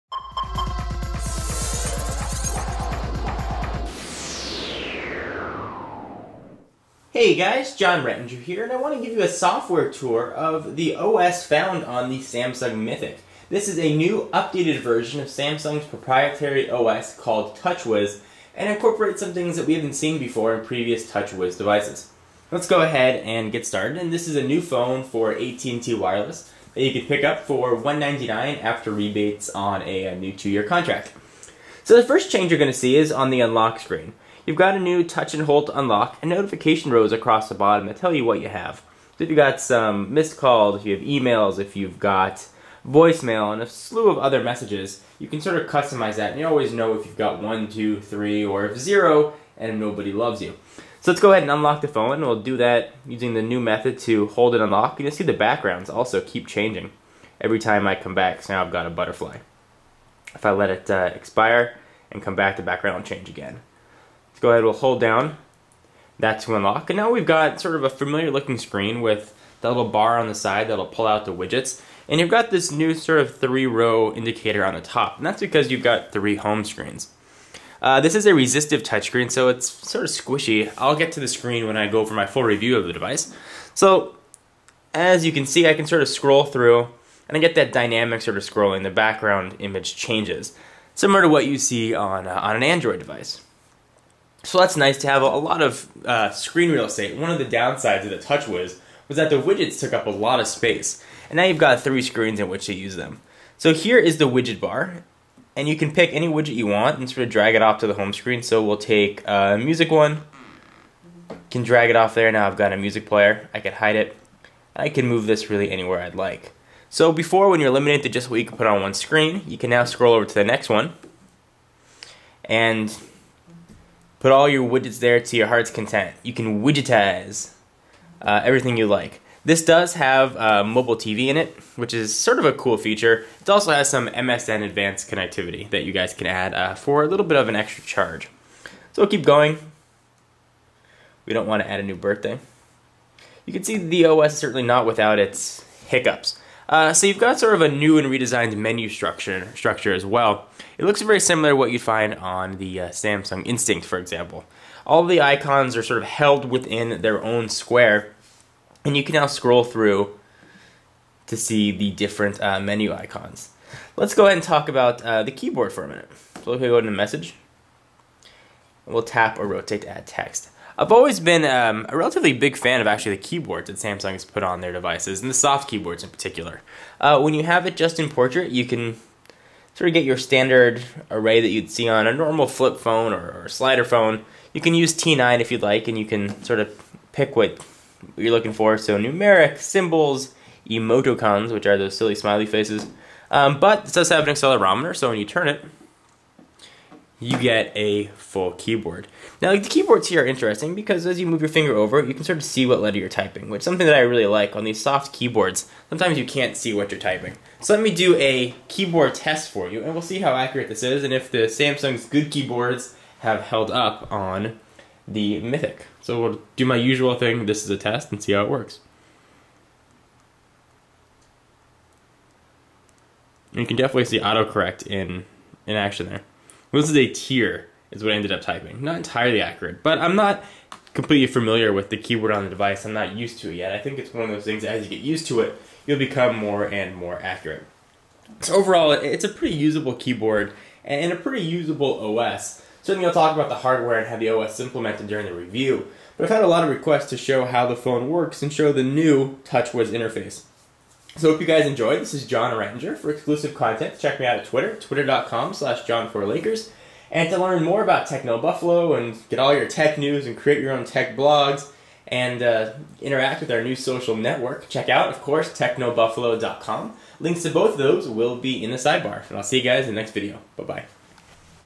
Samsung Mythic Software Tour
Giving a software tour of the new Samsung Mythic.